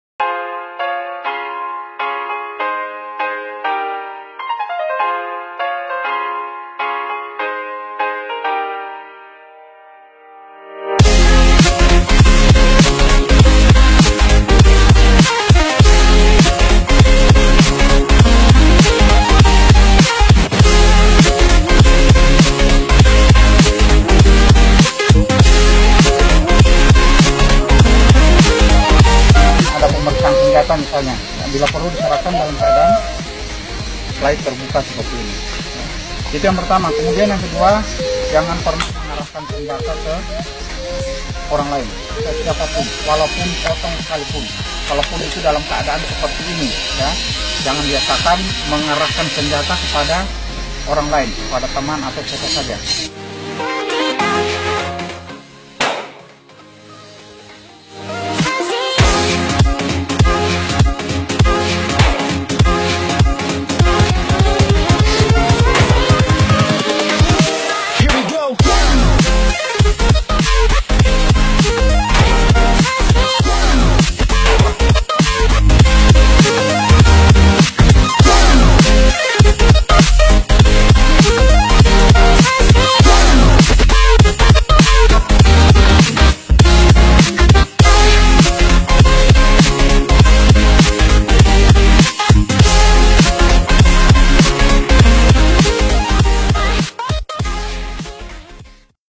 LATIHAN MENEMBAK MENGGUNAKAN SENJATA GENGGAM
PANEL OPERASIONAL

VLOG LATIHAN MENEMBAK DAN CARA MEMEGANG SENJATA OLEH POLRES BONE BOLANGO POLDA GORONTALO

BRAVO POLRI.
HABISKAN AMUNISI YANG ADA SEBELUM EKSPAIR...!!!